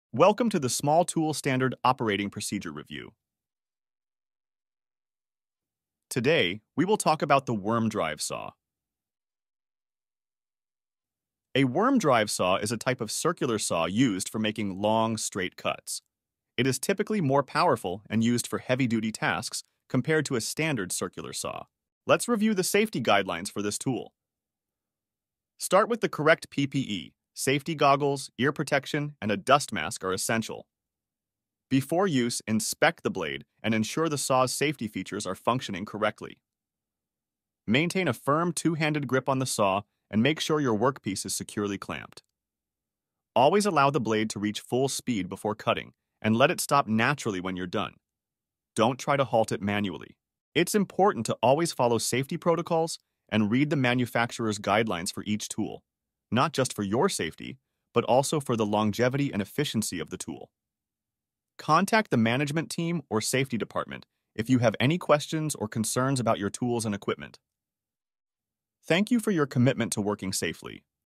Small Tool SOP # 22 - Worm Drive Saw
This training discusses the safe use of a Worm Drive Saw.